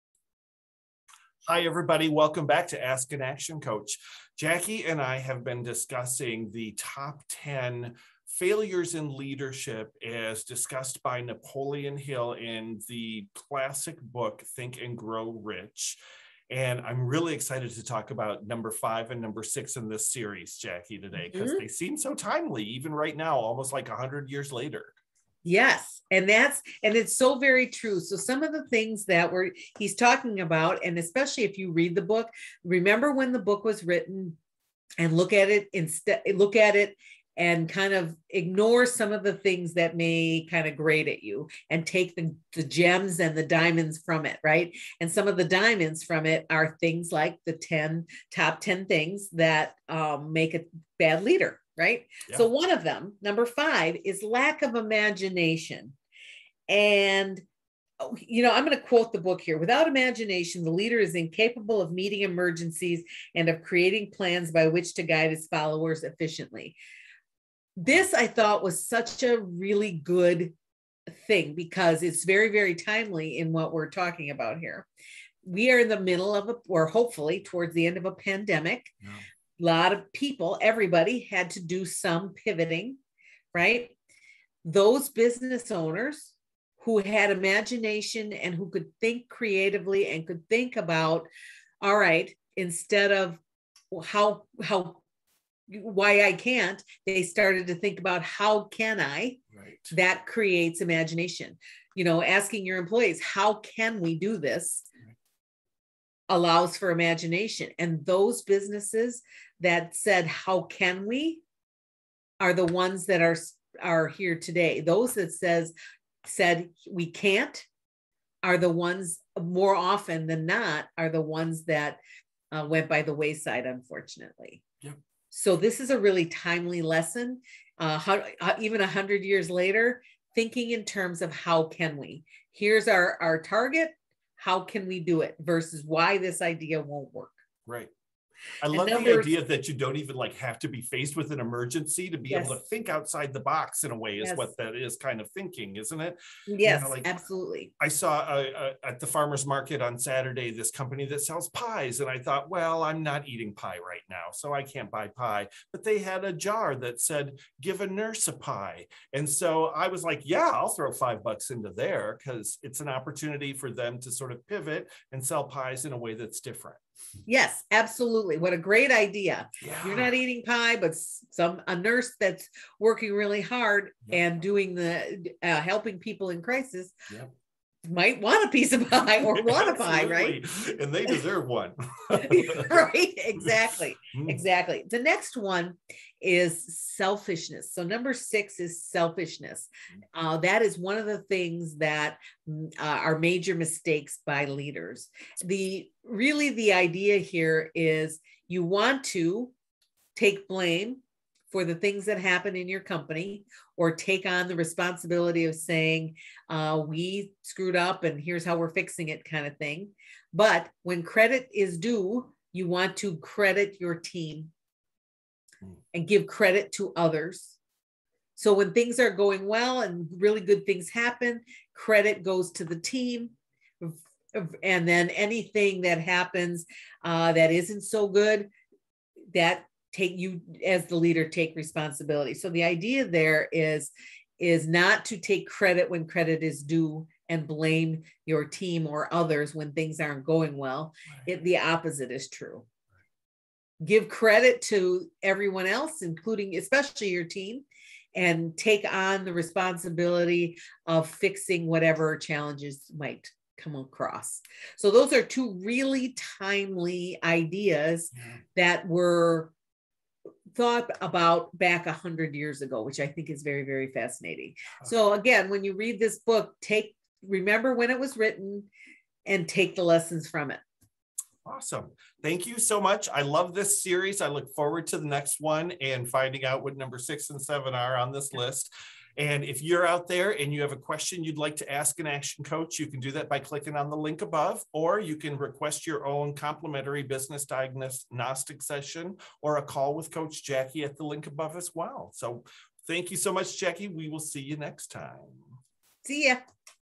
Ask An ActionCOACH: Ep 69 - Five and Six of the Ten Causes of Leadership Failure
ActionCOACH of Wisconsin's Joseph Muenich and Jackie Zach answer YOUR questions about what to do and how to run successful businesses!

This week:
-  Leadership
-  Lack of imagination
-  Selfishness